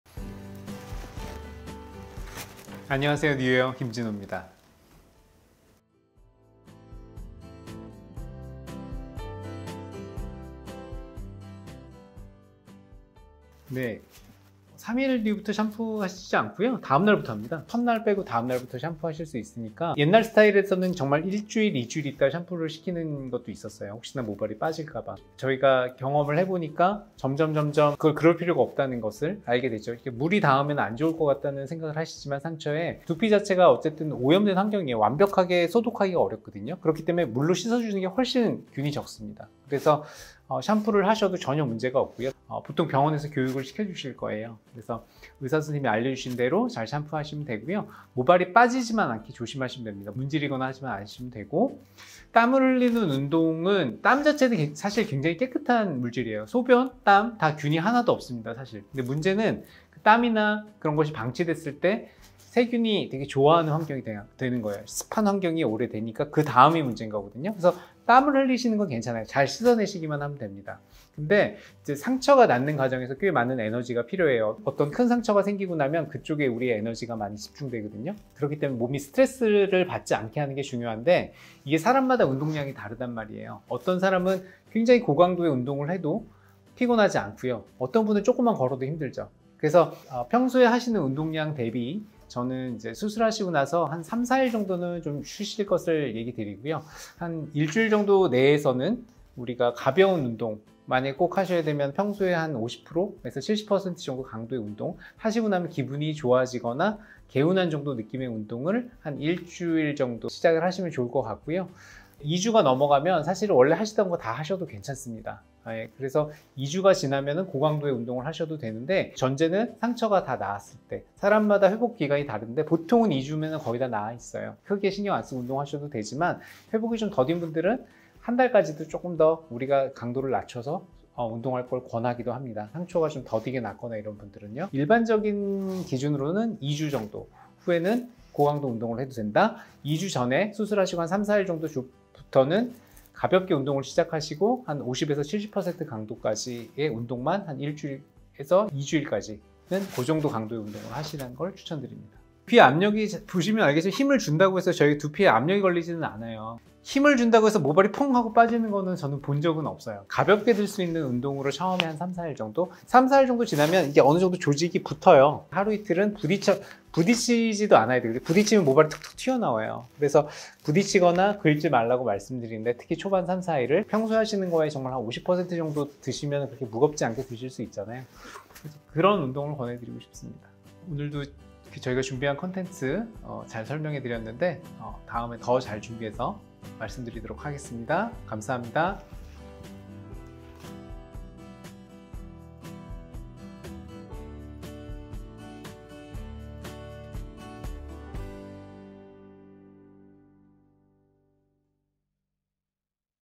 탈모병원 의사가 정확하게 알려주는 모발이식 주의사항｜뉴헤어
*3:10 수술기간 1년

모발이식 수술 후 주의사항이 궁금하신가요?
모발이식 후 샴푸 방법과 운동에 대해 모두 알려드립니다!